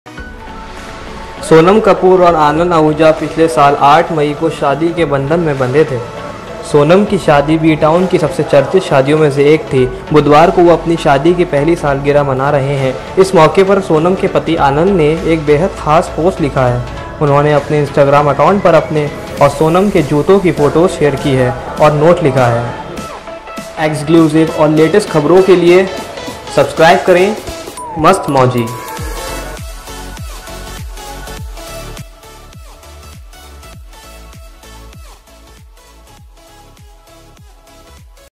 सोनम कपूर की शादी को एक साल हुआ पूरा | Mastmauji | EXCLUSIVE NEWS
सोनम को बताया मार्गदर्शक
आनंद लिखते हैं- मैंने हमारे जूतों की फोटोज इसलिए शेयर नहीं की क्योंकि मुझे जूते पसंद हैं। बल्कि इसलिए की है क्योंकि नीचे देखने से जमीन से जुड़े रहना याद रहता है। आगे वो लिखते हैं- 'आज हमारी शादी को 1 साल और हमारे रिश्ते को 3 साल हो चुके हैं। आपका जीवन साथी अगर आपका सबसे अच्छा दोस्त है तो इससे अच्छा कुछ और नहीं हो सकता है। एक ऐसा व्यक्ति आपकी लाइफ में होना चाहिए जो आपको आगे बढ़ने के लिए प्रेरित करता हो ये सबसे अच्छा अनुभव है। हैप्पी एनिवर्सरी टू माय सोनम कपूर, आप मेरी मार्गदर्शक स्टार हैं!'